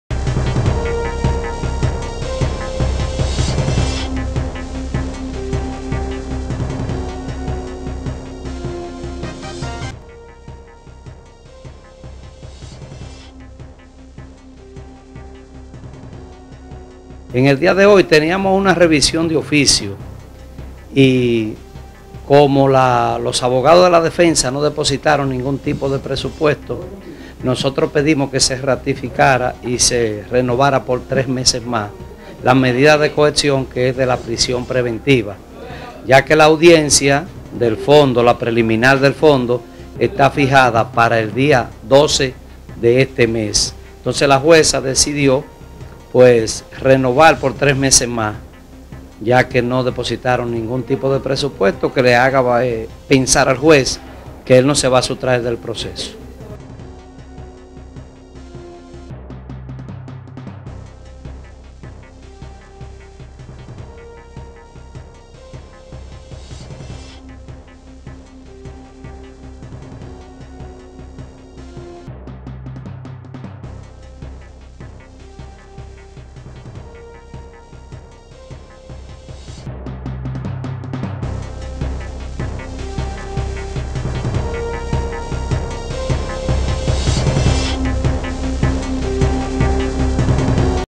Renuevan prisión preventiva al Sargento de PN acusado de asesinar dos jóvenes en SFM
El juzgado de la instrucción del distrito Judicial Duarte, renovó la medida de coerción impuesta en contra del sargento de la PN  Juan Cristian Pichardo de Aza por la muerte de dos jóvenes en esta ciudad.